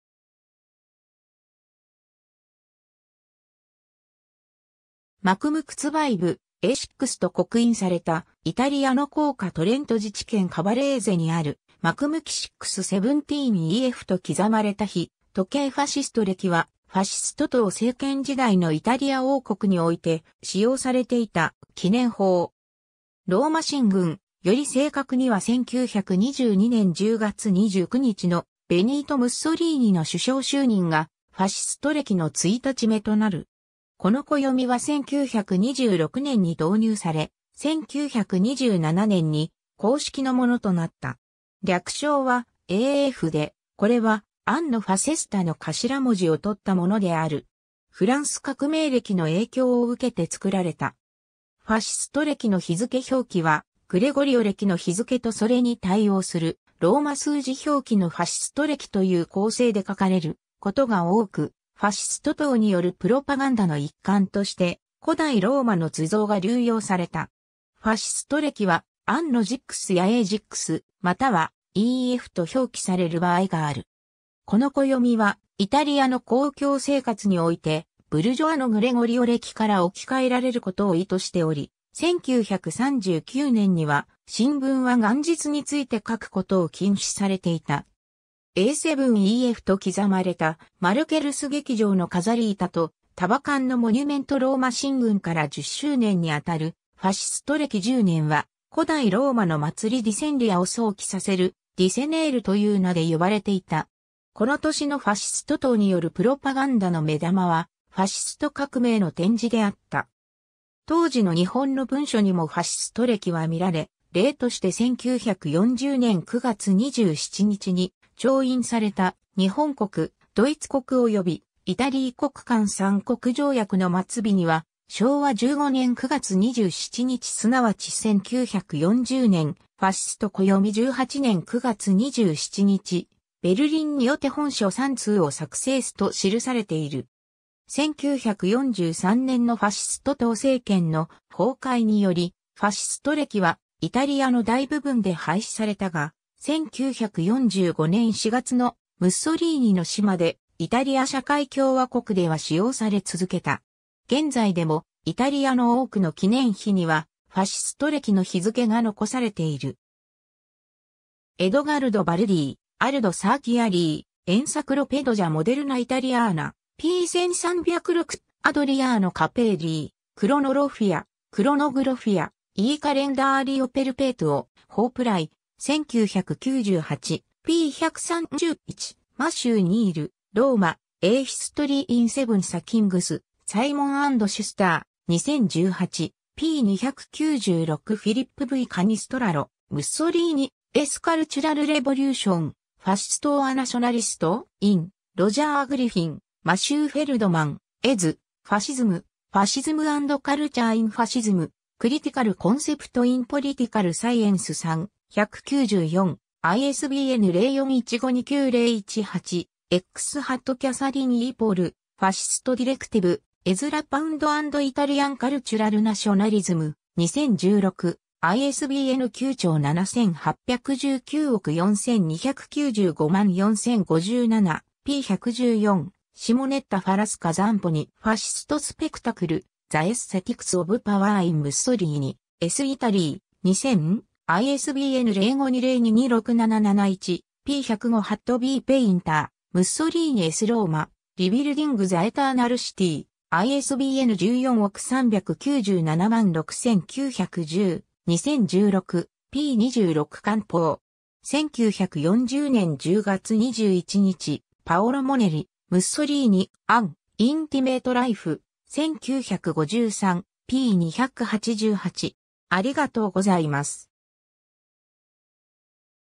ファシスト暦
MCMXXVIII A.VI（1928年・ファシスト暦6年）と刻印されたイタリアの硬貨 トレント自治県カヴァレーゼにある、MCMXXXIX XVII E F（1939年・ファシスト暦17年）と刻まれた日時計 ファシスト暦（ファシストれき、伊: Era Fascista）は、ファシスト党政権時代のイタリア王国において使用されていた紀年法。
 ローマ進軍、より正確には1922年10月29日のベニート・ムッソリーニの首相就任が、ファシスト暦の1日目となる。
この暦は1926年に導入され、1927年（ファシスト暦5年、Anno V）に公式のものとなった。
略称は "A.F." で、これは "Anno Fascista" の頭文字を取ったものである。
フランス革命暦の影響を受けて作られた。
 ファシスト暦の日付表記は、グレゴリオ暦の日付とそれに対応するローマ数字表記のファシスト暦という構成で書かれることが多く、ファシスト党によるプロパガンダの一環として、古代ローマの図像が流用された。
 ファシスト暦は、"Anno XIX" や "A. XIX"（ファシスト暦19年）、または "E.F." と表記される場合がある。
 この暦はイタリアの公共生活において、「ブルジョア」のグレゴリオ暦から置き換えられることを意図しており、1939年には、新聞は元日について書くことを禁止されていた。
 A. VII E.F.（ファシスト暦7年、Anno VII Era Fascista）と刻まれたマルケッルス劇場の飾り板と束桿のモニュメント ローマ進軍から10周年に当るファシスト暦10年（Anno X）は、古代ローマの祭り "Decennalia" を想起させる "Decennale" という名で呼ばれていた。
この年のファシスト党によるプロパガンダの目玉は、ファシスト革命の展示であった。
 当時の日本の文書にもファシスト暦は見られ、例として1940年（昭和15年、ファシスト暦18年）9月27日に調印された「日本國、獨逸國及伊太利國間三國條約」（日独伊三国間条約）の末尾には、「昭和十五年九月二十七日即チ千九百四十年、『ファシスト』暦十八年九月二十七日『ベルリン』ニ於テ本書三通ヲ作成ス」と記されている。
 1943年（ファシスト暦21年、Anno XXI）のファシスト党政権の崩壊により、ファシスト暦はイタリアの大部分で廃止されたが、1945年（ファシスト暦23年、Anno XXIII）4月のムッソリーニの死まで、イタリア社会共和国では使用され続けた。
 現在でもイタリアの多くの記念碑には、ファシスト暦の日付が残されている。
 ^ Edgardo Baldi, Aldo Cerchiari, Enciclopedia moderna italiana, p. 1306 ^ Adriano Cappelli, Cronologia, cronografia e calendario perpetuo, Hoepli, 1998, p. 131 ^ Matthew Kneale, Rome: A History in Seven Sackings, Simon and Schuster, 2018, p. 296 ^ Philip V. Cannistraro, "Mussolini's cultural revolution: fascist or nationalist?" in Roger Griffin, Matthew Feldman, eds., Fascism: Fascism and Culture in Fascism: Critical Concept in Political Science 3:194, ISBN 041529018X ^ Catherine E. Paul, Fascist Directive: Ezra Pound and Italian Cultural Nationalism, 2016, ISBN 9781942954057, p. 114 ^ Simonetta Falasca-Zamponi, Fascist Spectacle: The Aesthetics of Power in Mussolini's Italy, 2000, ISBN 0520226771, p. 105 ^ B. Painter, Mussolini's Rome: Rebuilding the Eternal City, ISBN ...